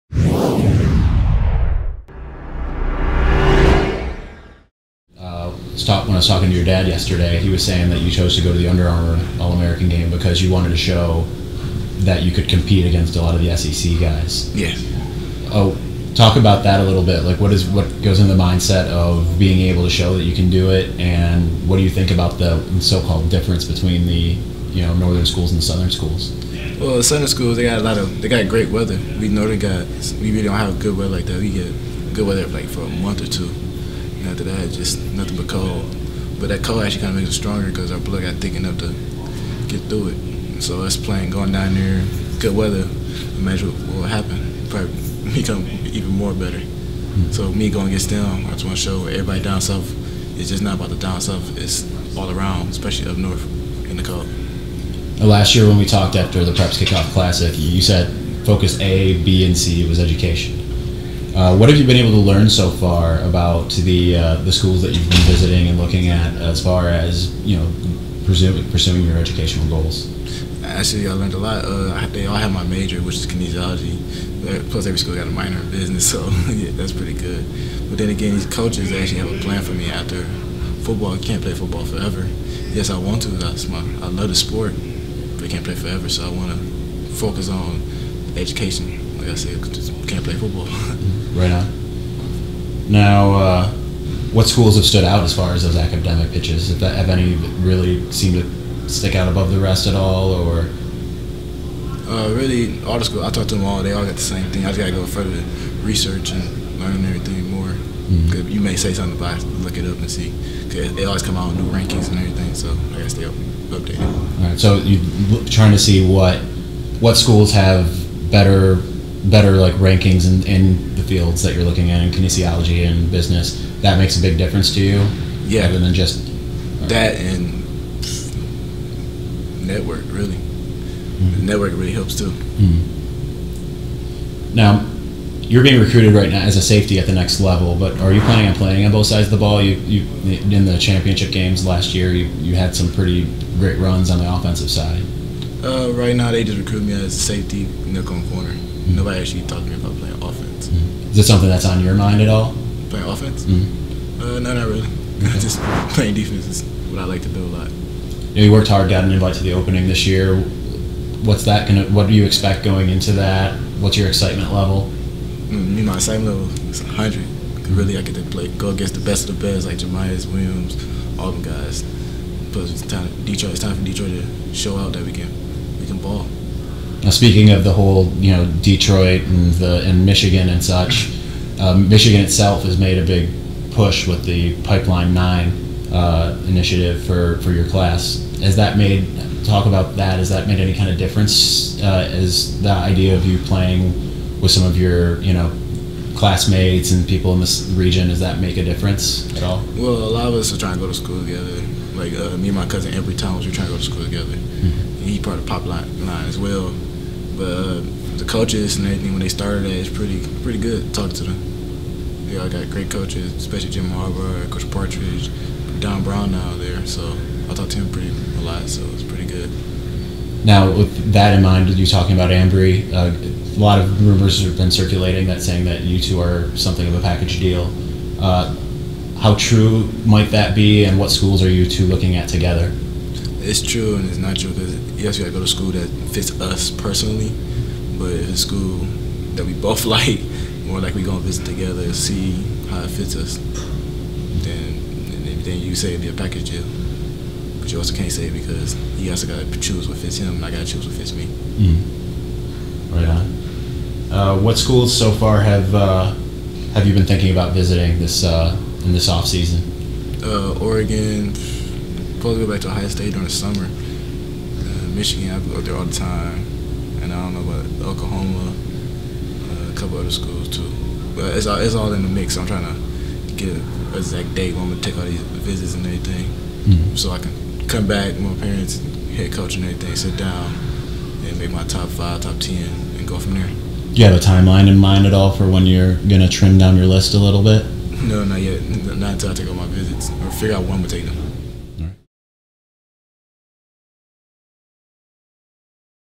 Cass Tech PART TWO: Jaylen Kelly-Powell
In a Wolverine247 exclusive, Detroit Cass Tech safety Jaylen Kelly-Powell talks about being a package deal with Ambry Thomas, why he chose the Under Armour All-American Game and how important education is to him and why.